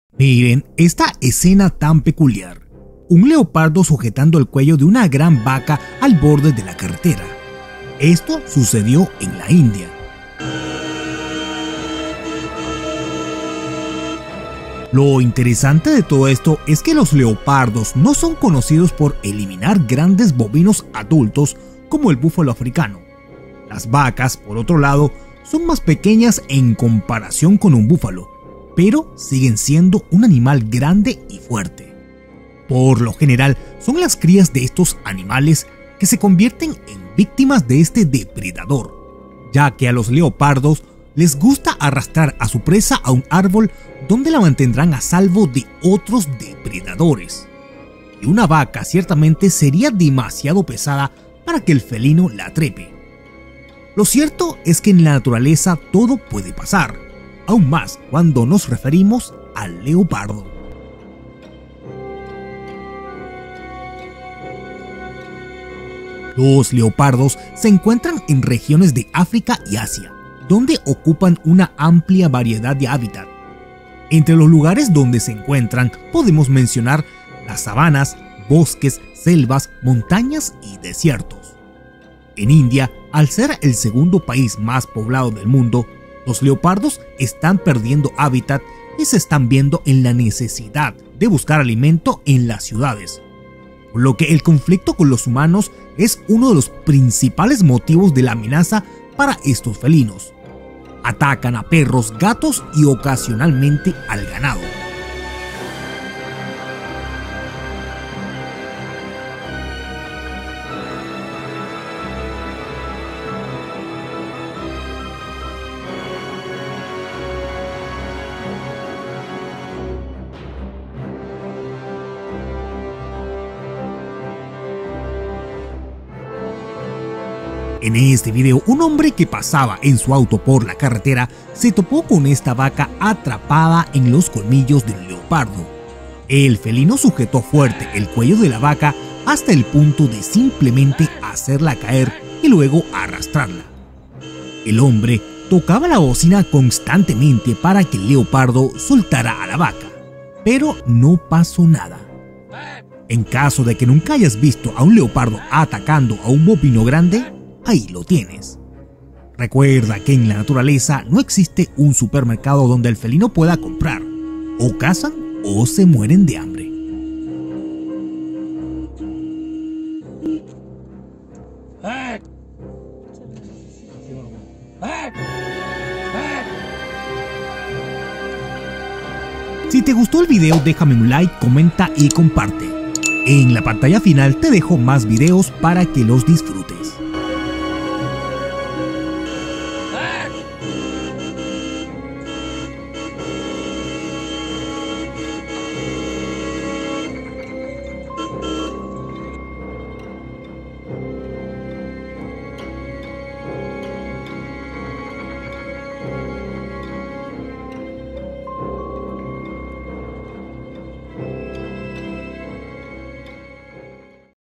¡La vaca no tuvo oportunidad! | LEOPARDO ANIQUILA A VACA EN LA CARRETERA
LEOPARDO ANIQUILA A VACA EN LA CARRETERA

Miren que escena tan peculiar: un leopardo sujetando el cuello de una gran vaca al borde de la carretera, esto sucedió en la India.

Lo interesante de todo esto es  que los leopardos no son conocidos por eliminar  grandes bovinos adultos como el búfalo africano. Las vacas, por otro lado, son más pequeñas en comparación con un búfalo, ¡pero siguen siendo un animal grande y fuerte!

 Por lo general, son las crías de estos animales  se convierten en víctimas de este depredador, ya que a los leopardos les gusta arrastrar a su presa a un árbol donde la mantendrán a salvo de otros depredadores. Y una vaca ciertamente sería demasiado pesada para que el felino la trepe,  lo cierto es que en la naturaleza todo puede pasar, más aún cuando nos referimos al leopardo.

Los leopardos se encuentran en regiones de África y Asia, donde ocupan una amplia variedad de hábitats. Entre los lugares donde se encuentran, podemos mencionar las la sabanas, bosques, selvas, montañas y desiertos. En India, al ser el segundo país más poblado del mundo, los leopardos están perdiendo hábitat y se están viendo en la necesidad de buscar alimento en las ciudades, por lo que el conflicto con los humanos es uno de los principales motivos de la amenaza para estos felinos. Atacan a perros, gatos y ocasionalmente al ganado.

En el video, un hombre que pasaba en su auto por la carretera, se topó con esta vaca atrapada en los colmillos de un leopardo. El felino sujetó fuerte el cuello de la vaca  hasta el punto de simplemente hacerla caer y luego arrastrarla. El hombre tocaba la bocina constantemente para que el leopardo soltara a la vaca. ¡Pero no paso nada! En caso de que nunca hayas visto a un leopardo atacando a un bovino grande, ¡ahí lo tienes! Recuerda que en la naturaleza no existe un supermercado donde el felino pueda comprar. ¡O cazan, o se muere de hambre!

Esto es Top Mas, Tu canal de Entretenimiento.